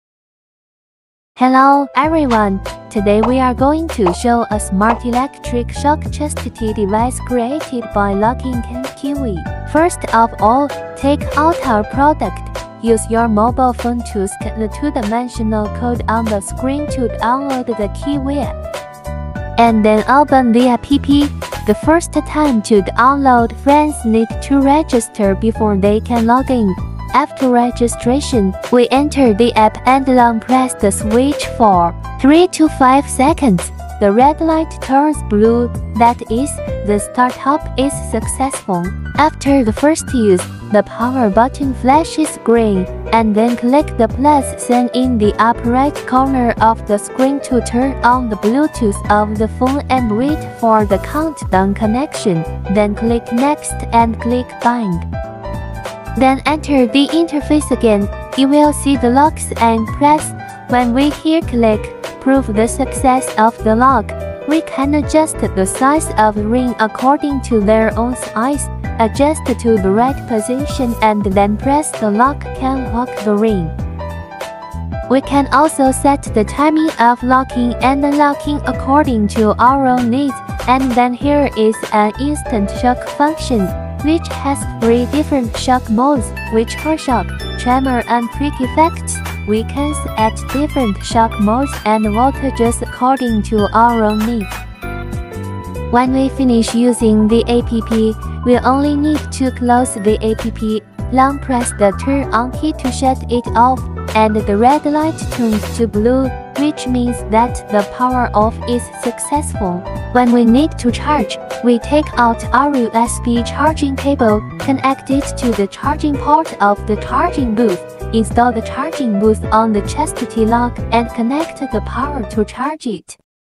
MChastity - Cellmate v2 app controlled chastity cage ，How to use Cellmate V2
The QIUI's cellmate chastity cage takes chastity play to the next level with its cloud-controlled electrostimulation feature. The user can be both excited and terrified, anticipating the arrival of their master while fearing the electric punishment that can be delivered at any time. With the timed electroshock function, the user can set an alarm to deliver electric shocks at specific intervals, just like setting an alarm clock.

You can take advantage of YouTube exclusive discounts code: YTB12%OFF